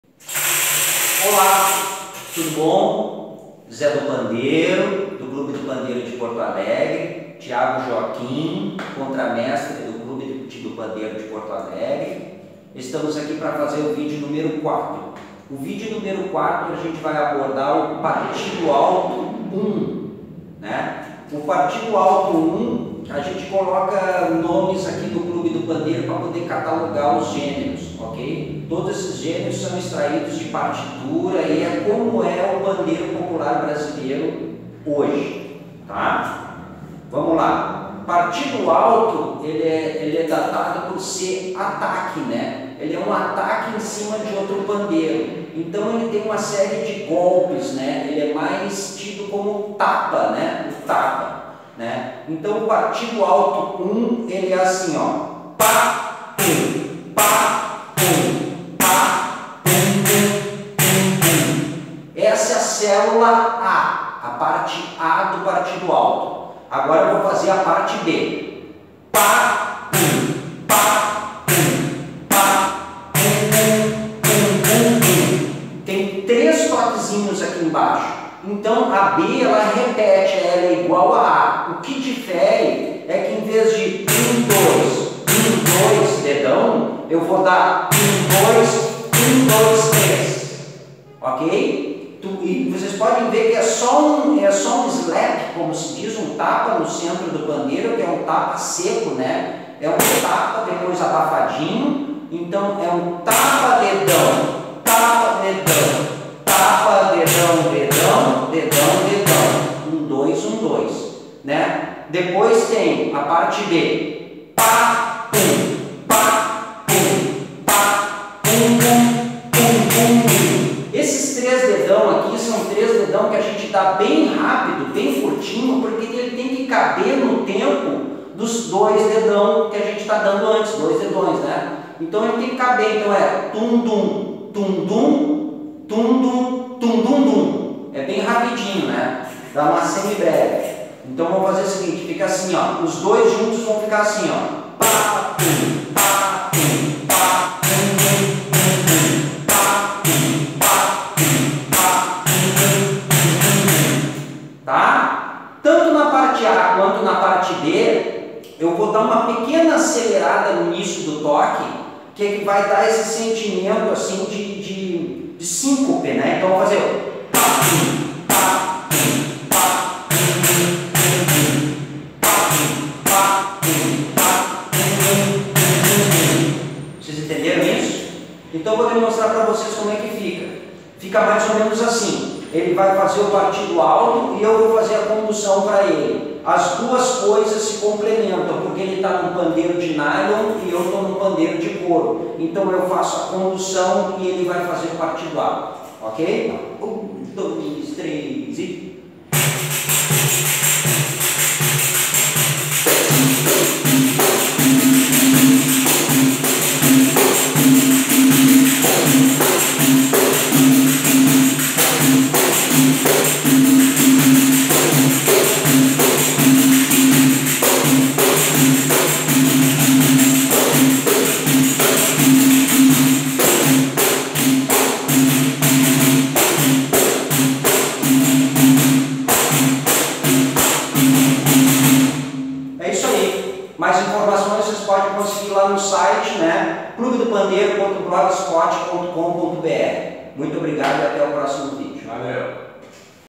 [ 5 ] PARTIDO ALTO 1 - AULA DE PANDEIRO com Zé do Pandeiro
Aula Prática de Pandeiro com Zé do Pandeiro e Thiago Joaquim
SAMBA DE PARTIDO ALTO 1.